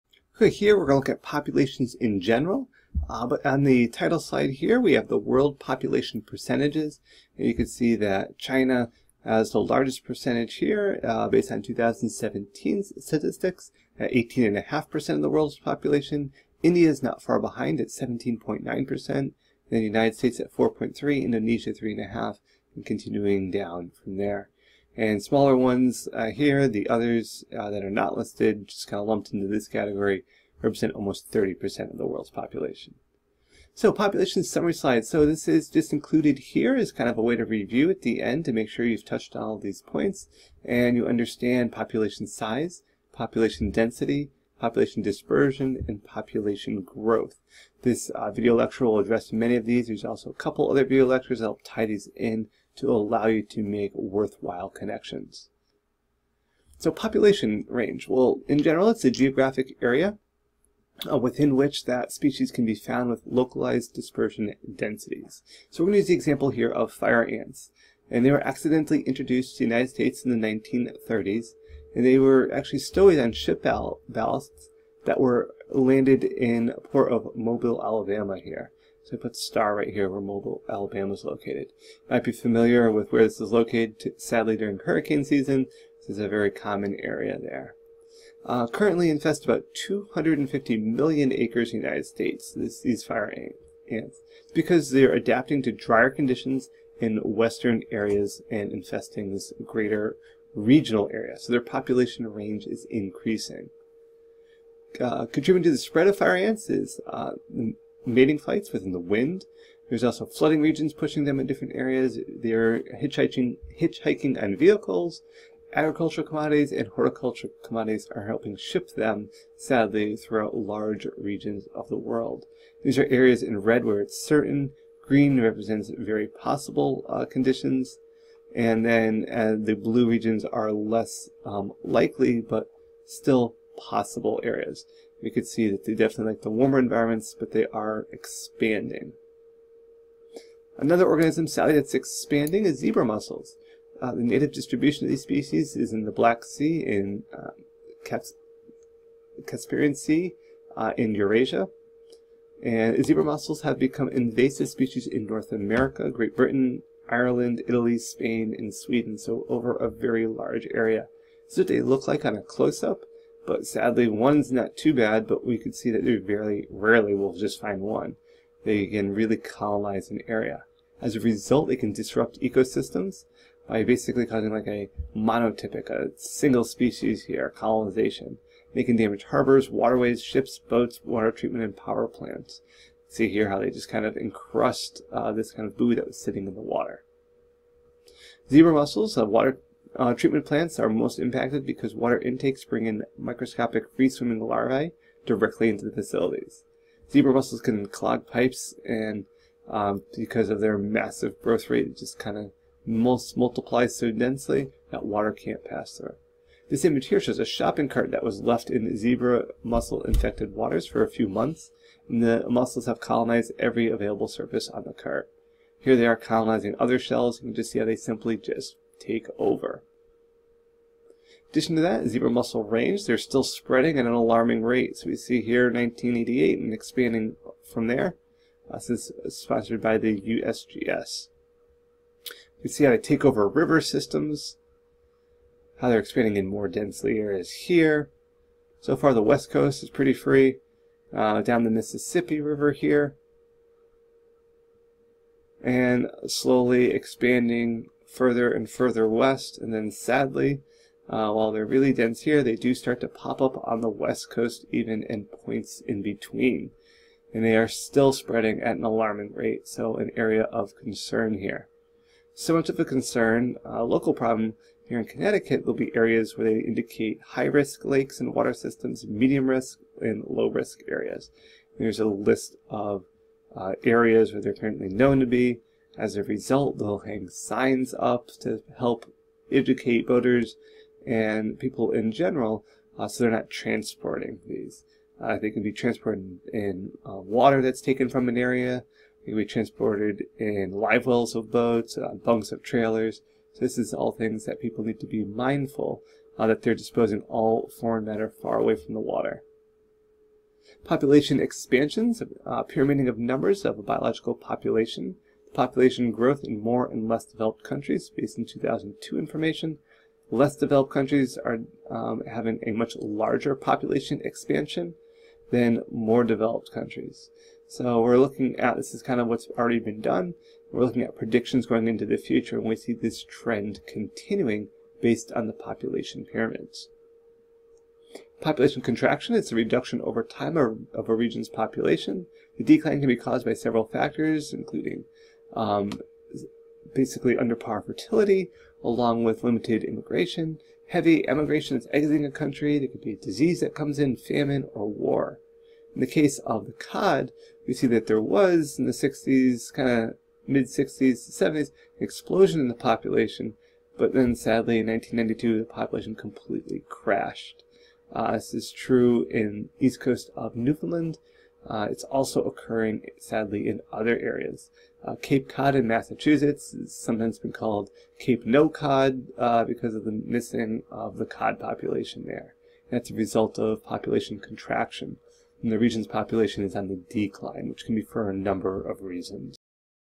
Populations
A population is a group of individuals of the same species living together.

Critical properties of a population include...

1. Population size- The number of individuals in a population.

2. Population density- Population size per unit area.

3. Population dispersion- Scatter of individuals within a population’s range.

4. Population growth- How populations grow and the factors affecting growth.

*Due to the description character limit the full work cited for "Populations" can be viewed